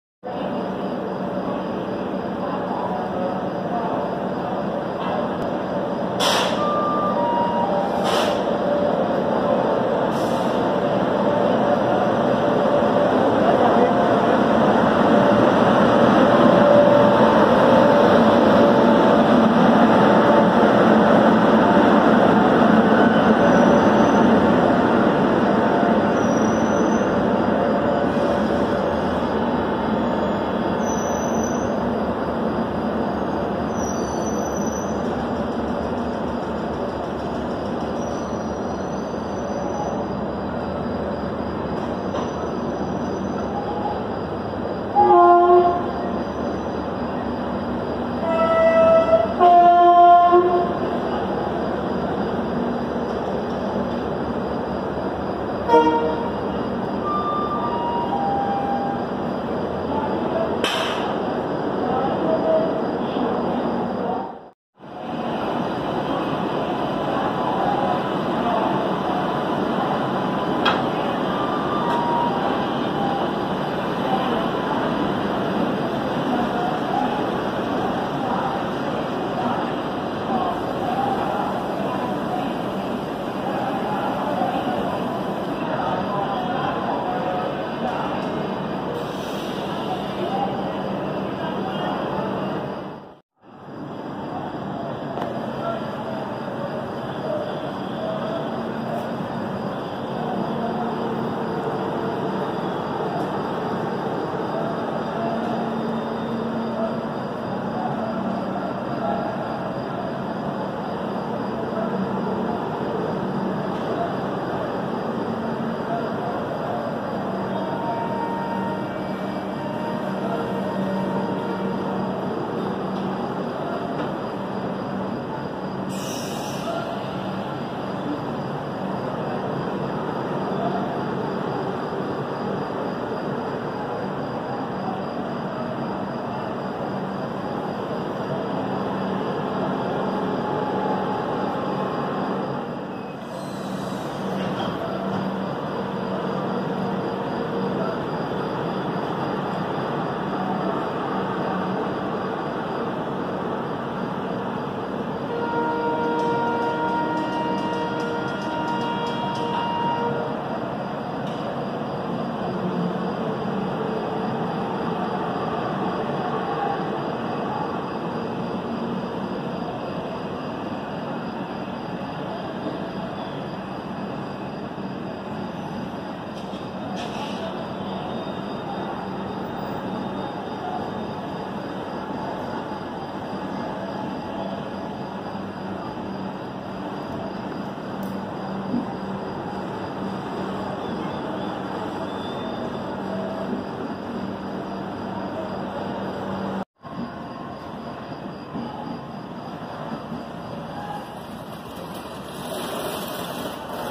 KANPUR CENTRAL: Arrival & Departure | 02314 New Delhi - Sealdah Rajdhani Special
02314 New Delhi - Sealdah Rajdhani Special hauled by HWH WAP-7 30523 arriving at Kanpur Central for its scheduled halt.